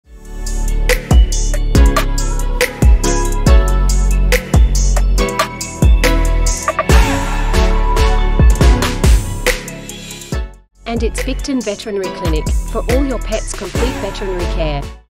2023-10-09
🎬 In this video, catch Sophie, the stunning Standard Schnauzer, getting her routine check-up.
Is your pet receiving our "Complete Veterinary Care"?

🌟 New clients get $10 off!

We look forward to meeting you and your pets at Bicton Veterinary Clinic.